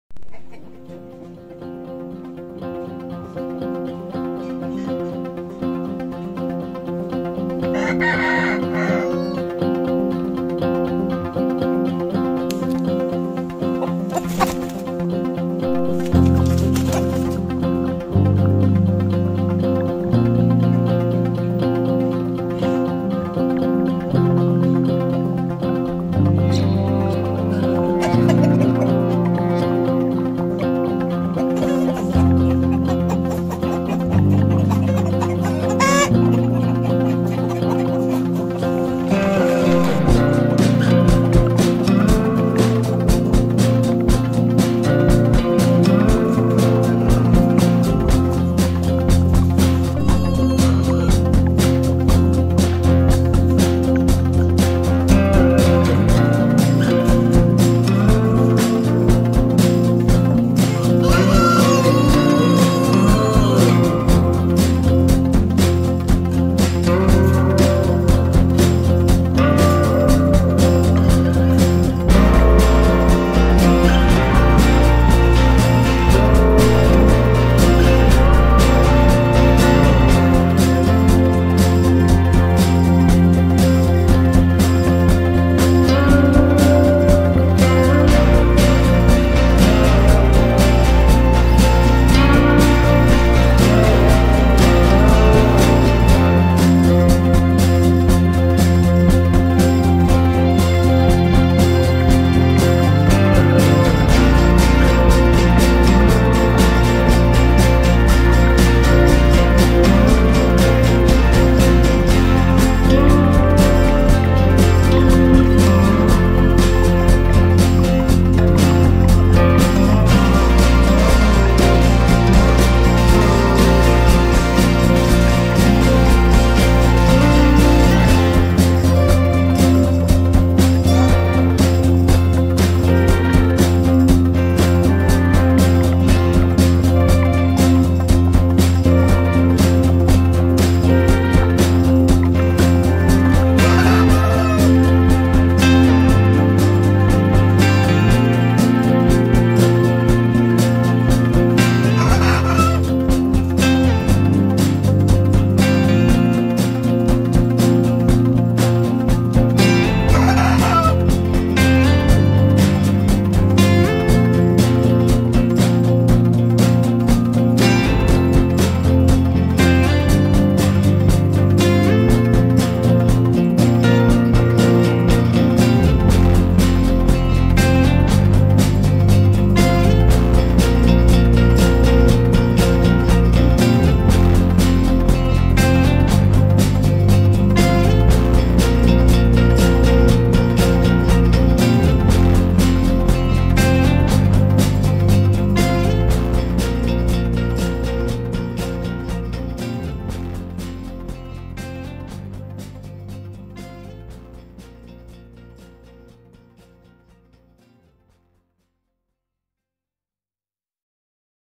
Dong Tao Chicken: Unique Features and Traditional Flavors of Vietnam
Dong Tao chicken, originating from Dong Tao village, is a traditional breed of Vietnamese chicken. Their peculiarity is not only their strong appearance, but also in the excellent quality of meat.